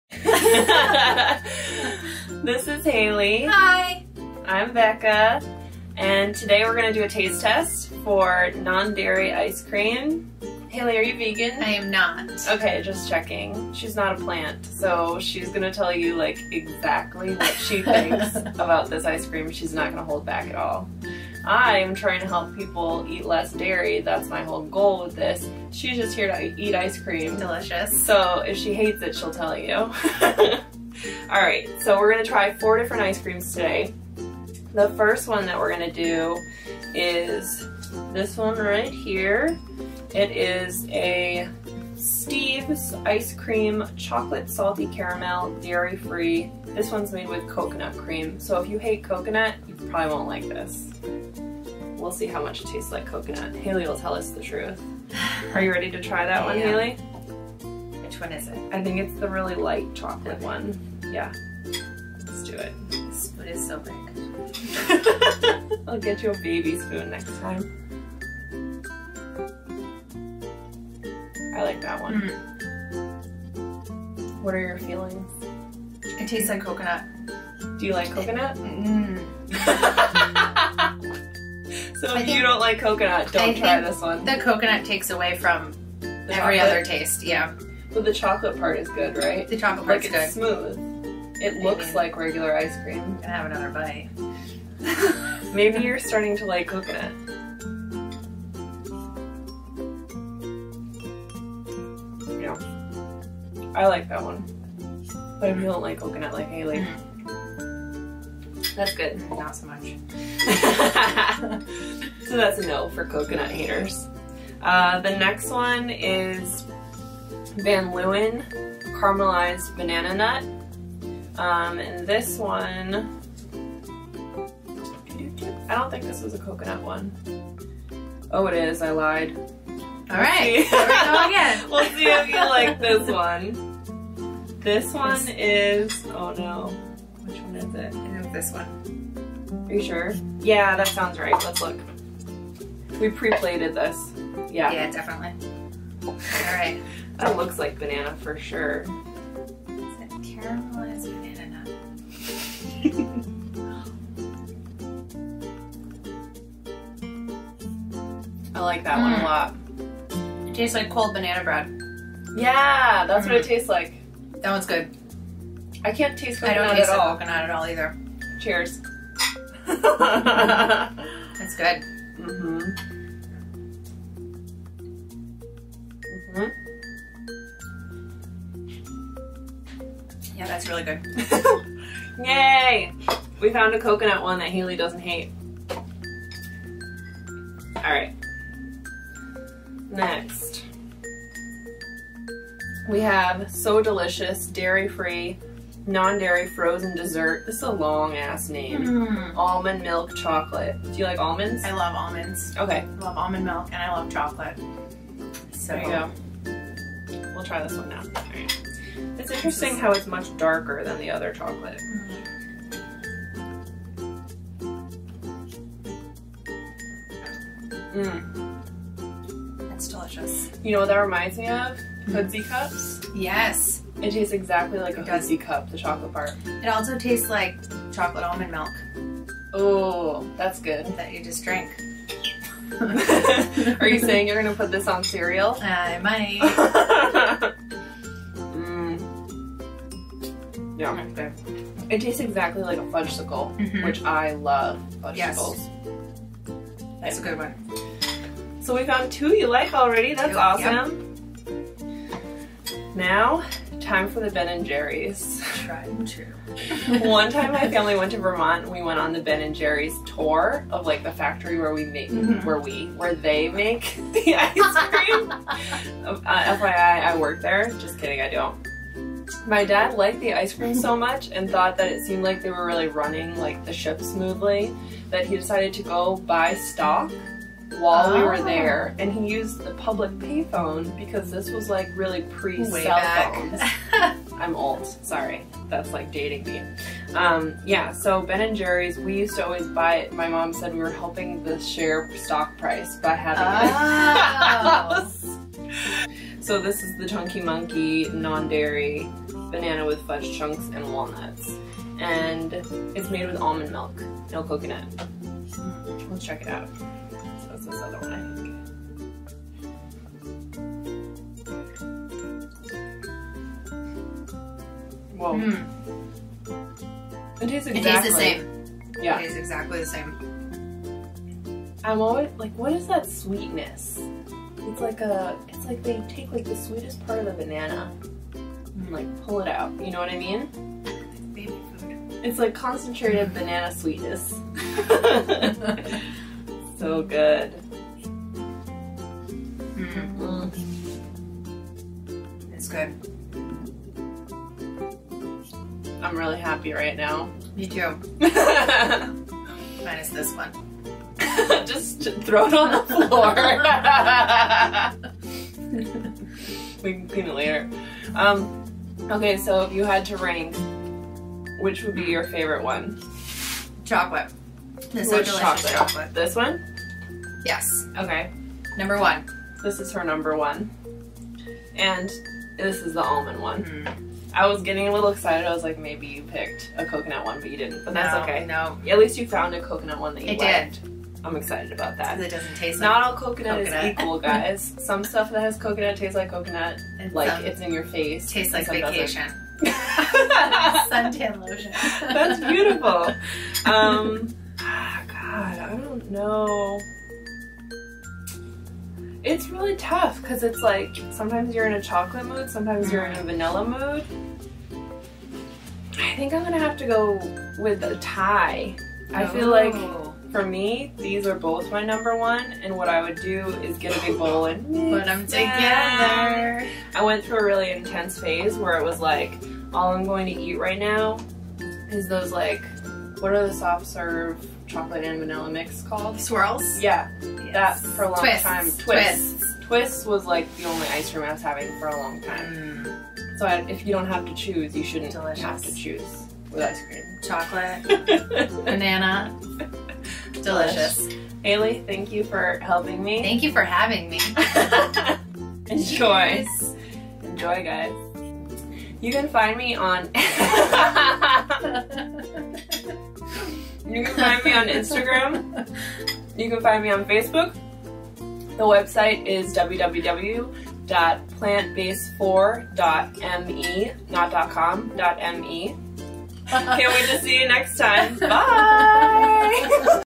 Is vegan ice cream for jerks?
We taste tested four vegan ice creams just for you!! Neither of us are vegan. for more goodies.